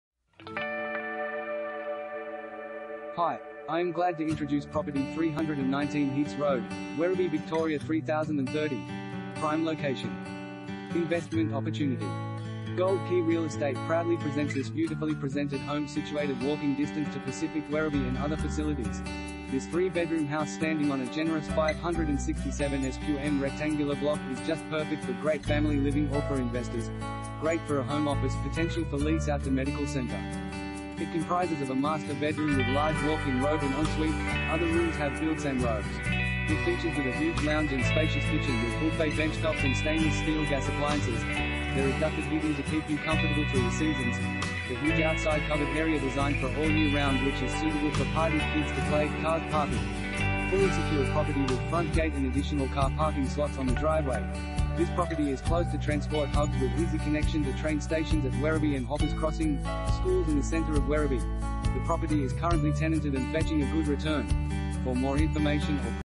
319 Heaths Road, Werribee VIC 3030 | AuPropertyReport.Com
319 Heaths Road, Werribee VIC 3030
Property Type: house
Bedrooms: 3
Bathrooms: 3
Car Space: 2
Prime Location! Investment opportunity!